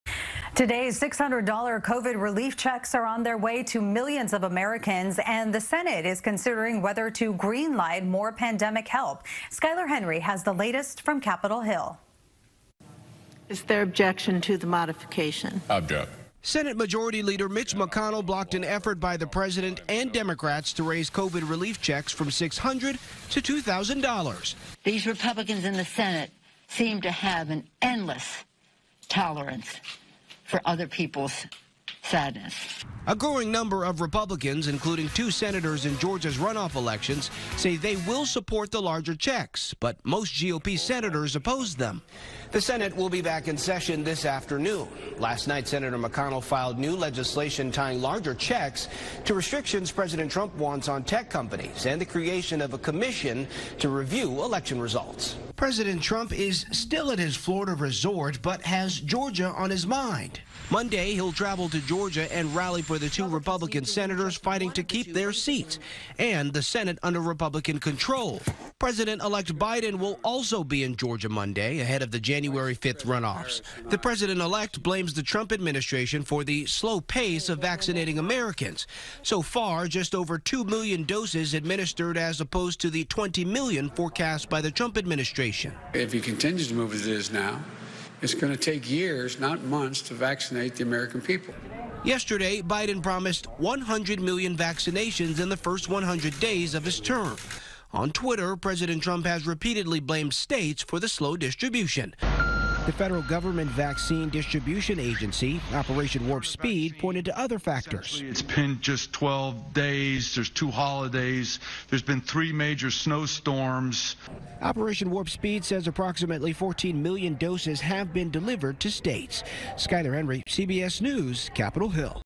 Senate Continues Debate Over Raising COVID Relief Check To $2,000
Skyler Henry reports on continued debate over raising amount of COVID stimulus checks to American people (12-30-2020)